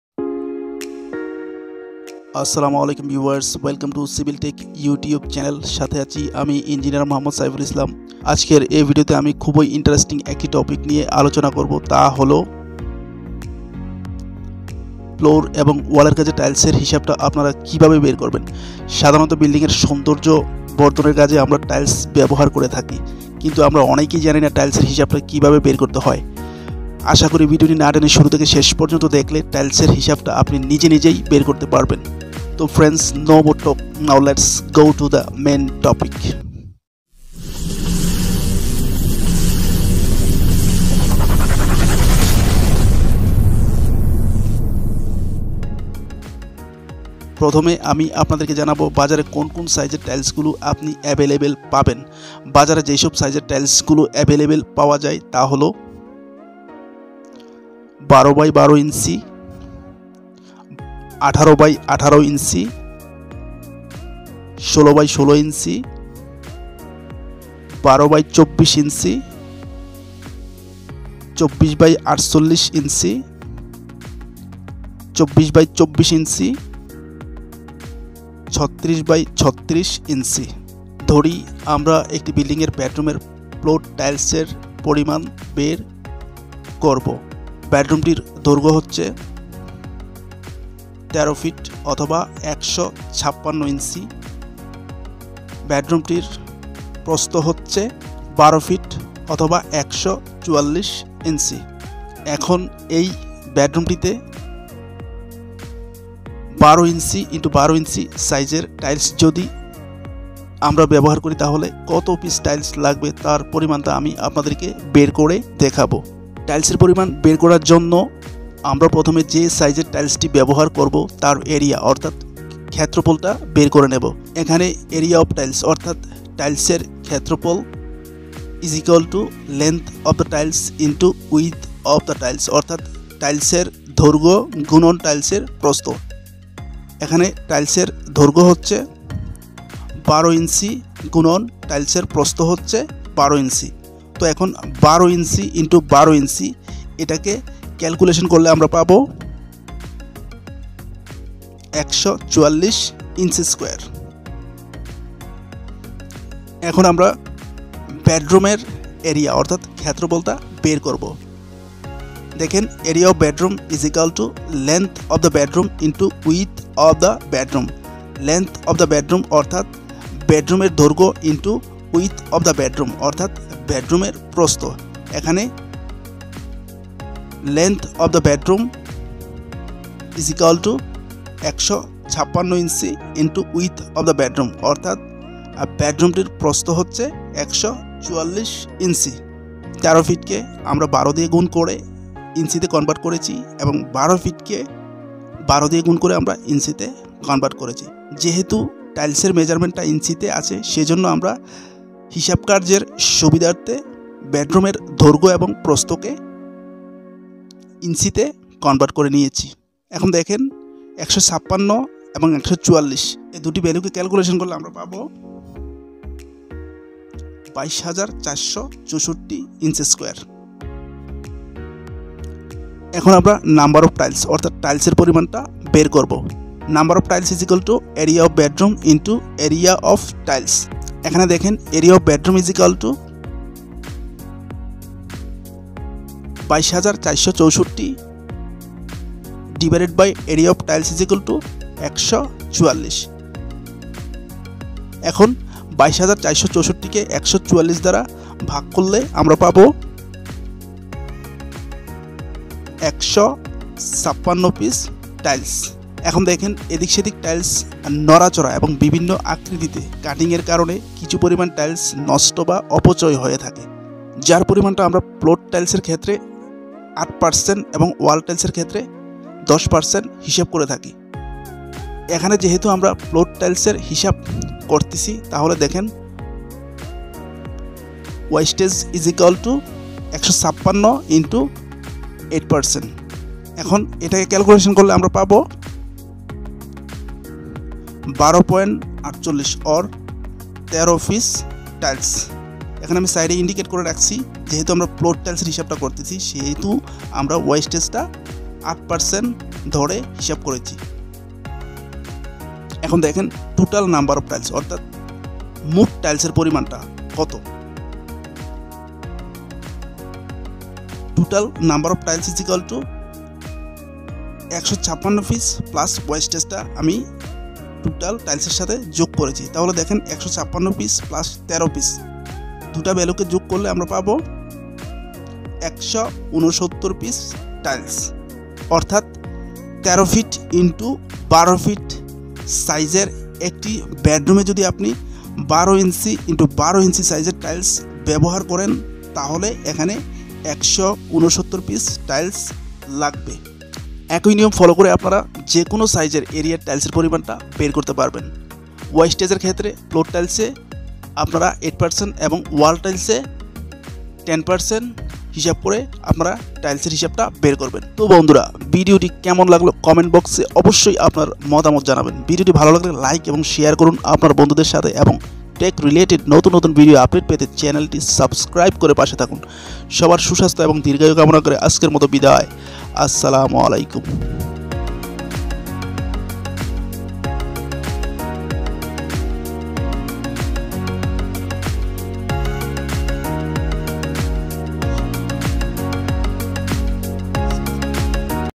Calculate floor and wall tiles||ফ্লোর এবং ওয়ালের কাজে টাইলসের হিসাব
CIVIL TECH
টাইলস আমরা সাধারণত ভবনের সৌন্দর্যবর্ধক কাজে ব্যবহার করে থাকি।। কিন্তু আমরা অনেকেই জানিনা ফ্লোর বা ওয়ালের কাজে টাইলসের হিসাব  কিভাবে বের করতে হয়। আশা করি যারা জানেন না তারা ভিডিওটি দেখলে খুব উপকৃত হবেন।এই ভিডিওর মাধ্যমে আমি দেখিয়েছি ফ্লোর টাইলস এর ক্ষেত্রে কত পার্সেন্ট অপচয় ধরবেন এবং ওয়াল টাইলস এর ক্ষেত্রে কত পার্সেন্ট অপচয় ধরবেন।